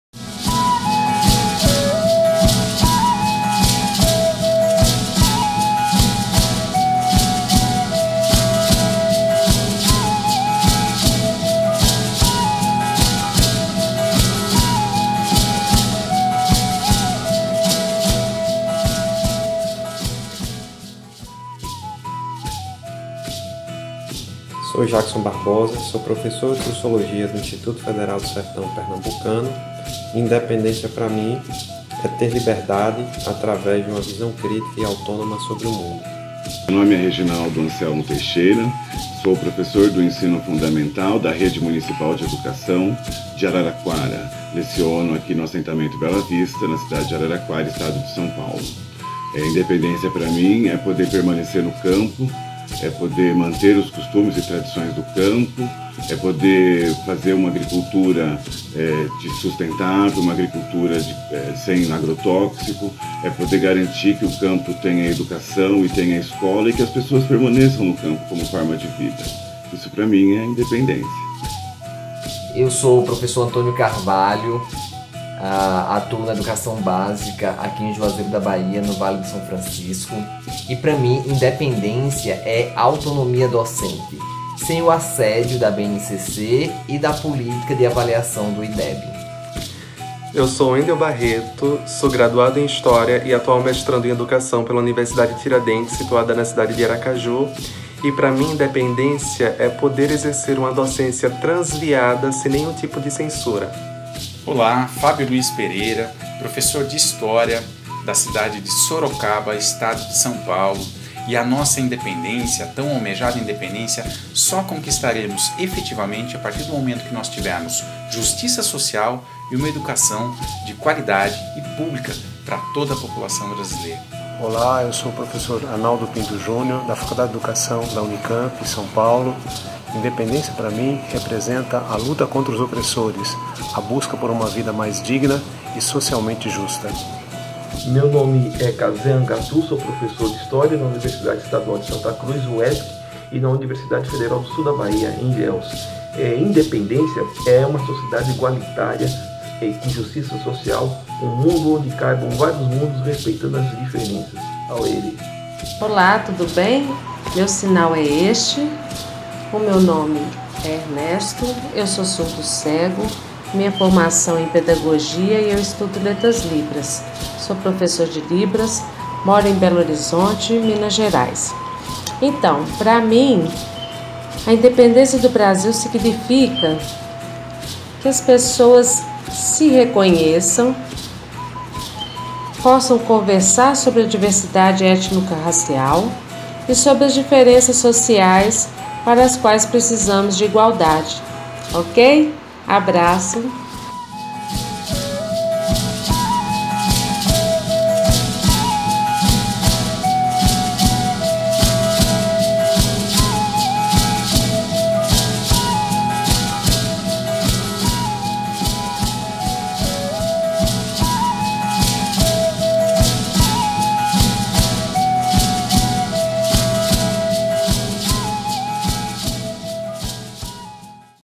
Parte 1 - SOMOS MUITOS
O Portal do Bicentenário apresenta:

"Somos muitos"!

Este é o primeiro vídeo da série “Brasileiros e brasileiras, iguais e diferentes, somos muitos e muitas!”. Nele, professores de diferentes partes do Brasil falam sobre suas compreensões sobre a Independência e enfatizam a importância da justiça social, da autonomia, da resistência à censura e das lutas em defesa da educação pública, dignidade, diversidade étnico-racial e acessibilidade!

Parte 2- Somos Muitas;
Parte 3- Diferentes e Iguais;
Parte 4 - Brasileiros e Brasileiras.